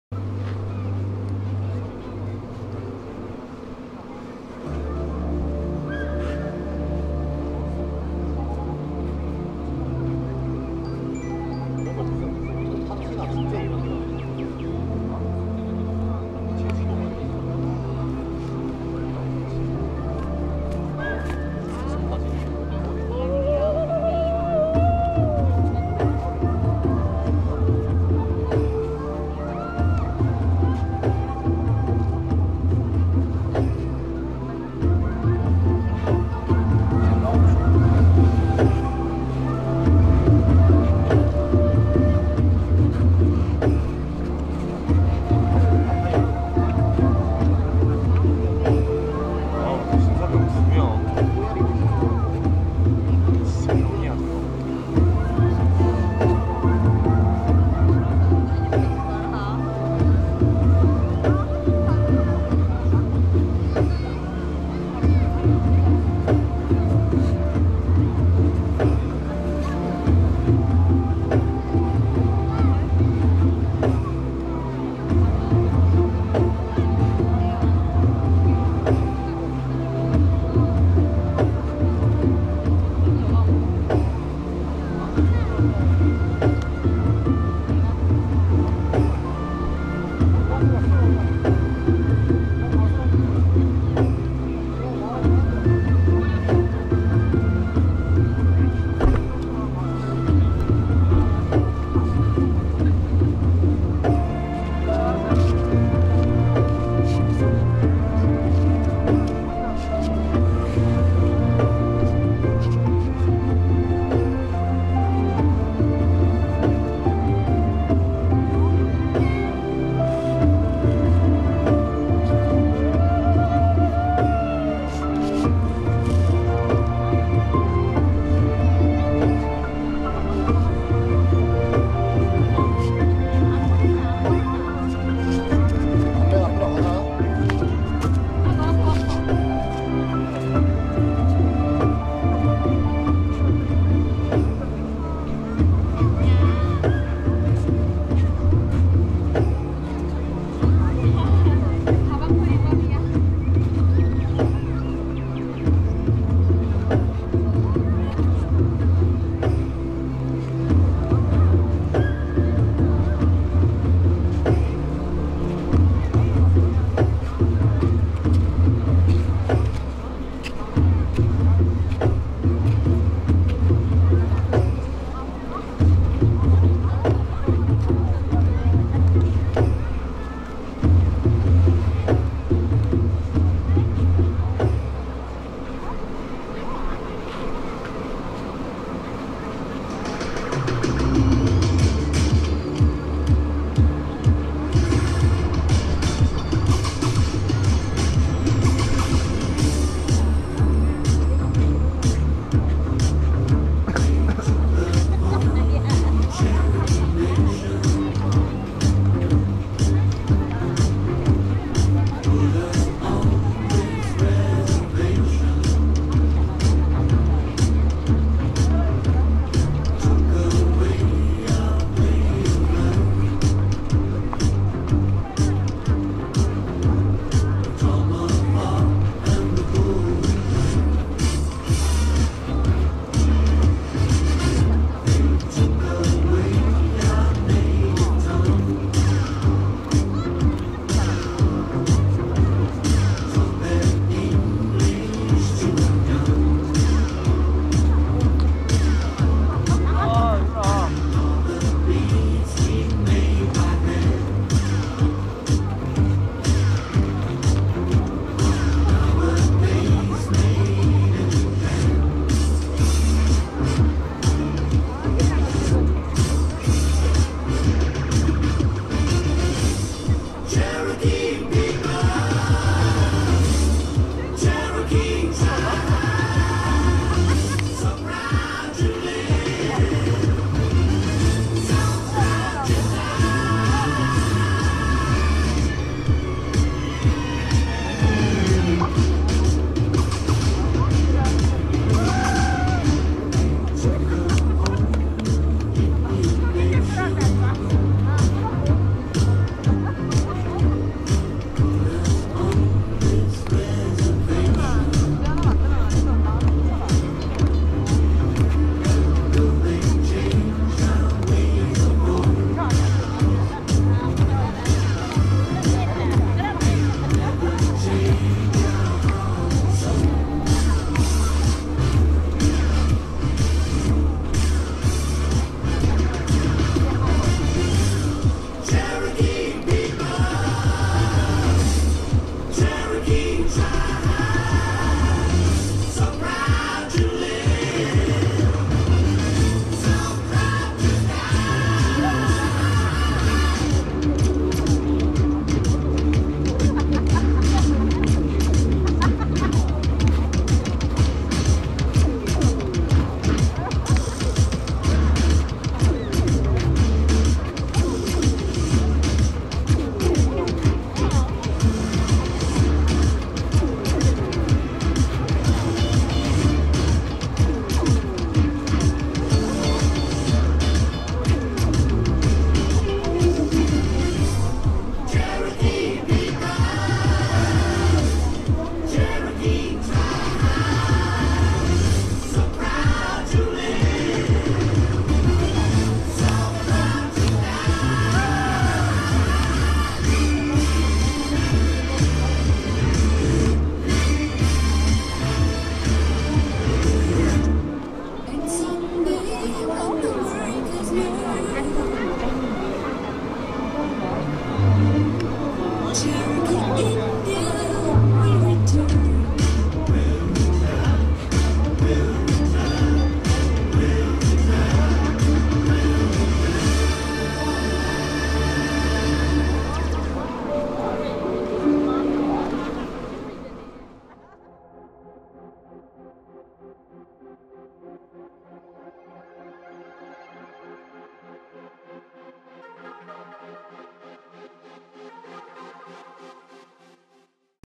해운대 버스킹_백성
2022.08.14. 해운대해수욕장에서 저녁 산책길에 만난 조금 어설프지만 특이한 "백성"님의 버스킹 영상 입니다.